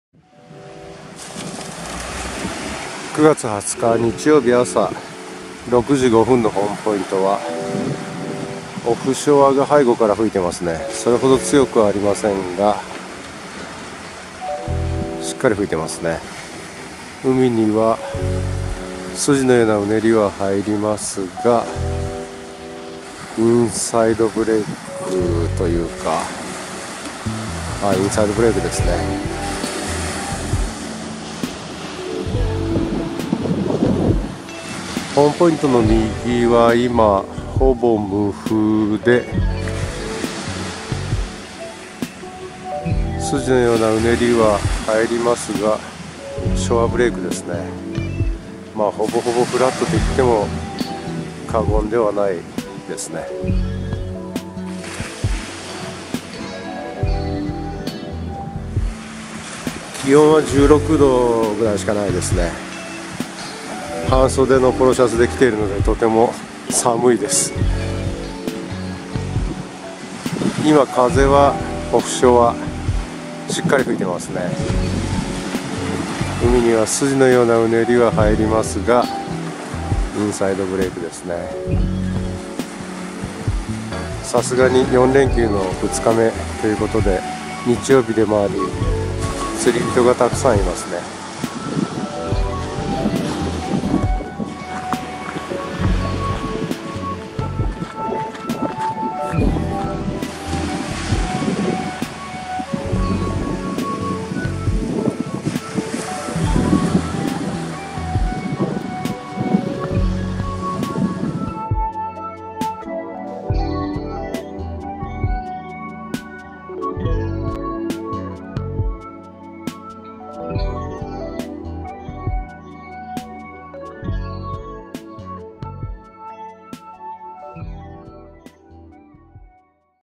オフショアでスジの様なウネリがインサイドブレイクのホームポイント 200920 ~サーフモンキーTV
今朝のホームポイントは気温16℃、オフショアでスジのようなウネリがインサイドブレイクです。釣り日和なのか既に9人がいます。天気予報によると今日は9時から終日、北北西風3～4mということで、少しは波が上がるかも…。よろしければどうぞ☝️